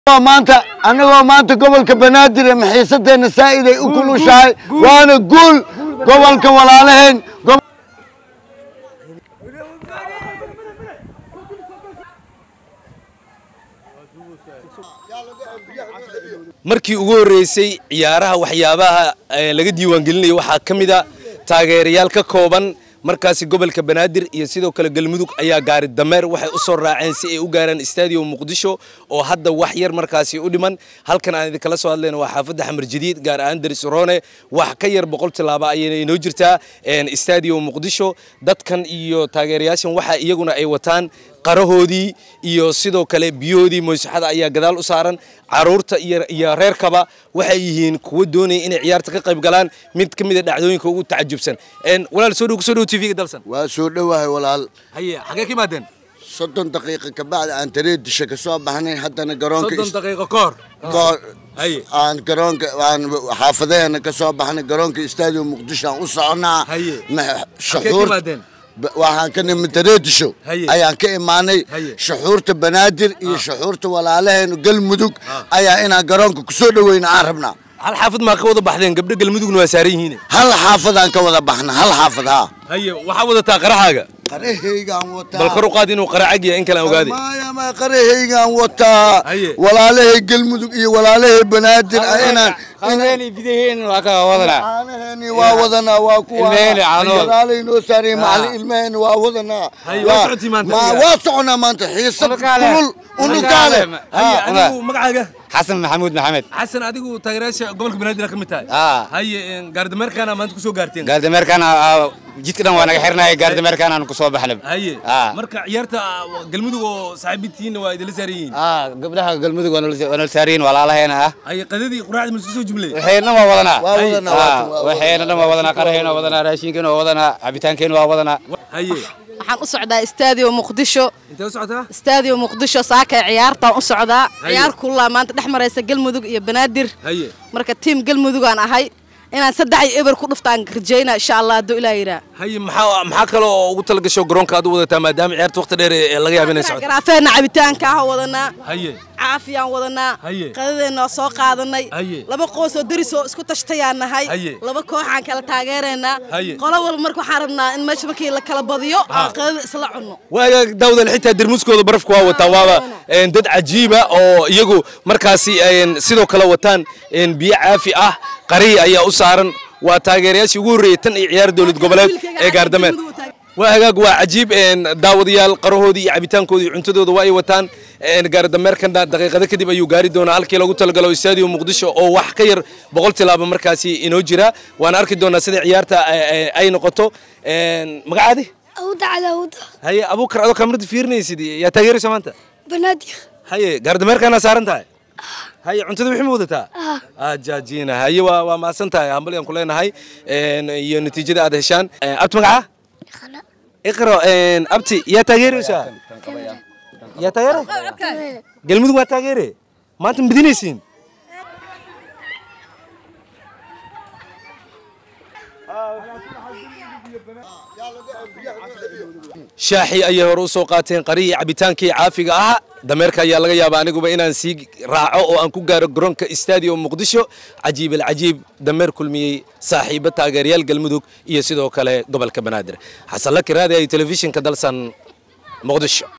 Gaari Dameer kulmiyayey taageerayaasha xulalka G/Banaadir iyo Galmudug iyagoo ku dhow afaafka hore
Gaari Dameer kulmiyayey taageerayaasha xulalka G/Banaadir iyo Galmudug iyagoo ku dhow afaafka hore ee Garoonka Stadium Muqdisho.